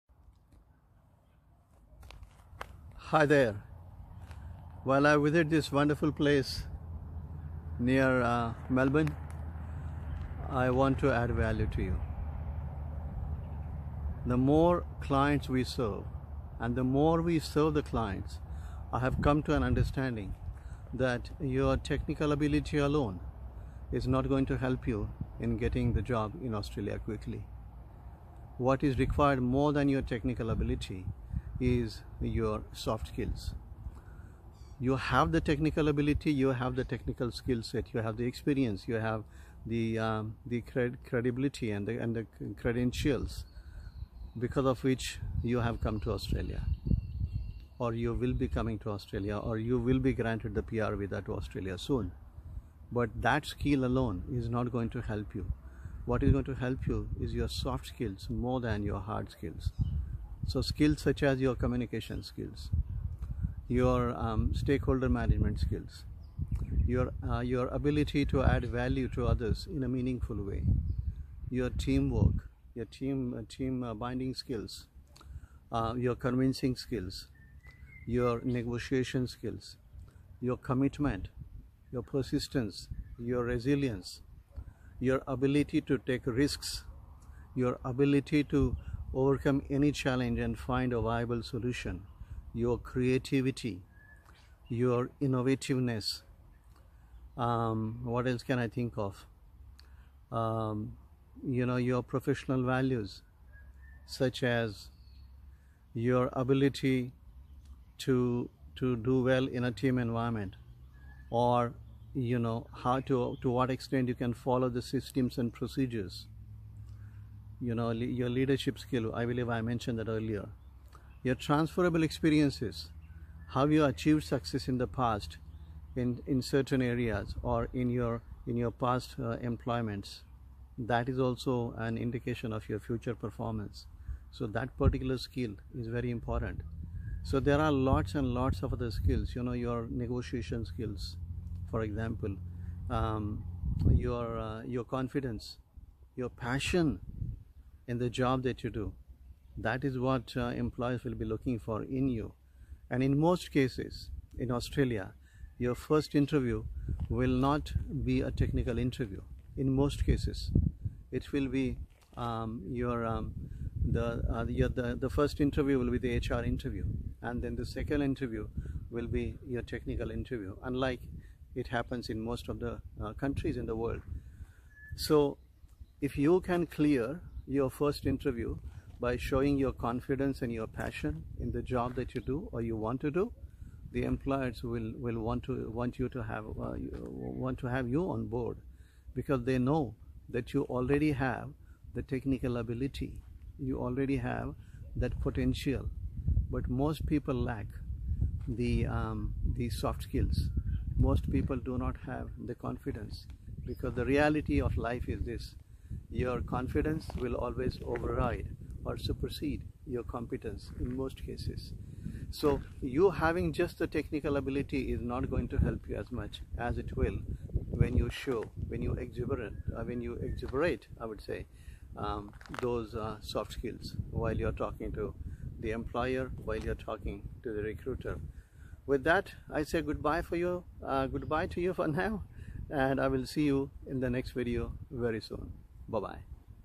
Jobs in Australia with soft skills: Career Launch Australia
Discover why your soft skills are critical to your job search in Australia